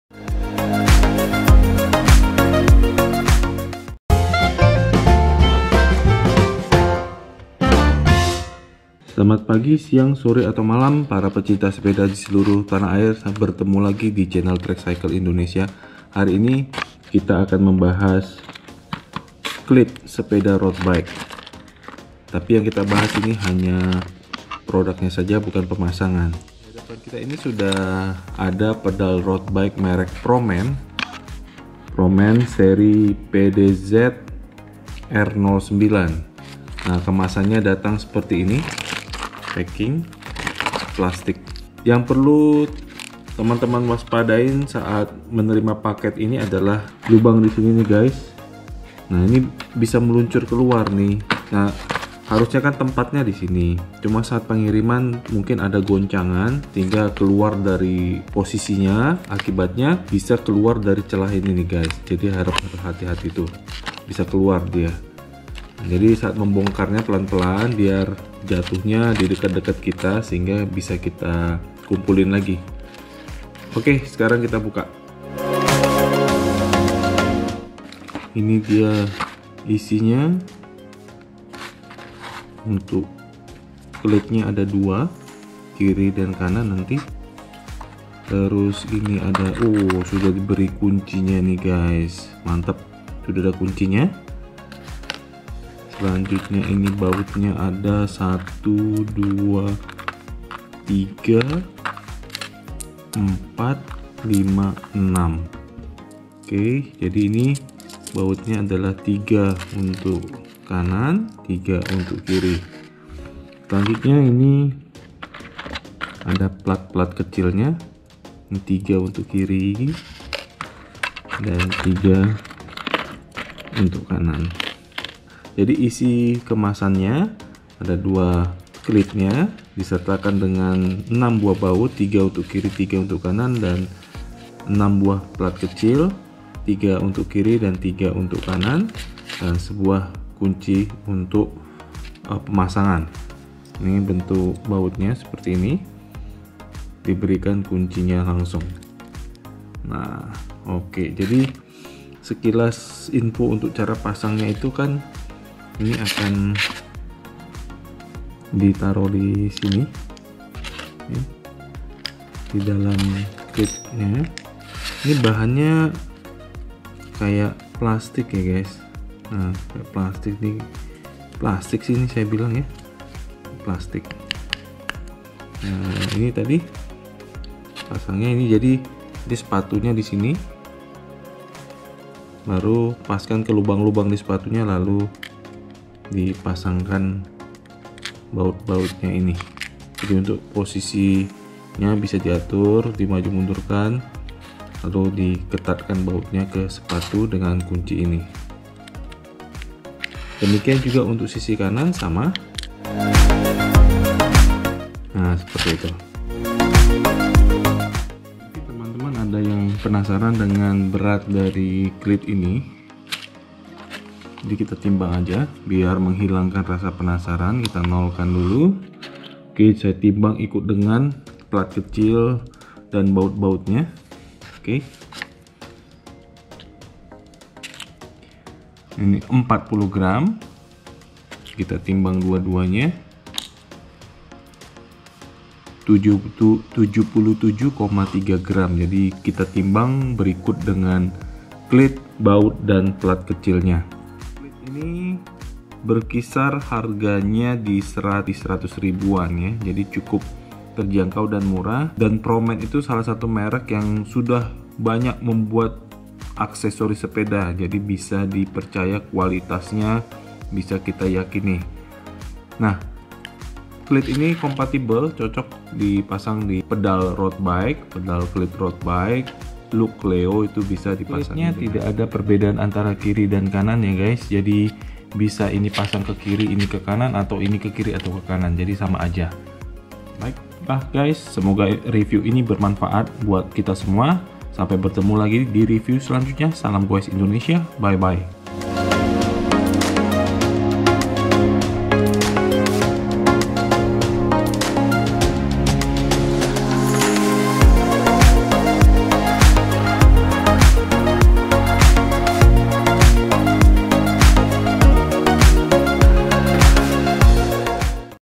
Review Cleat Roadbike Murah Berkualitas Untuk Look Keo - Promend PDZ R09
Cleat  Promend PDZ R09 merupakan cleat untuk pedal roadbike, dan hanya compatible dengan pedal roadbike Look Keo. ( tidak compatible dengan pedal roadbike Shimano / SPD ).
Untuk informasi lebih lanjut apa saja isi paket pembeliannya, bahan cleat dan beratnya ... Yuk saksikan videonya sampai habis :)

Salam gowes Indonesia.

Kontak channel Trexcycle : trexcycleindonesia at gmail dot com